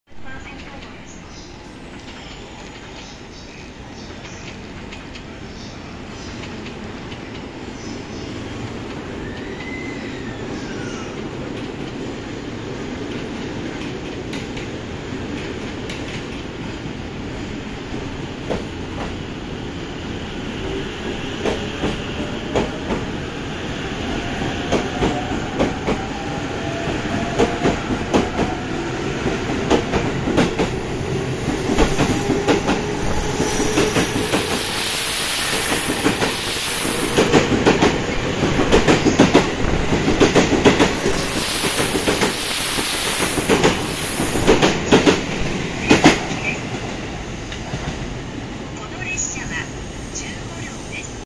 E257系 NC-33編成 NA-02編成 特急湘南12号東京行が大船駅2番線を発車するシーン
8:37大船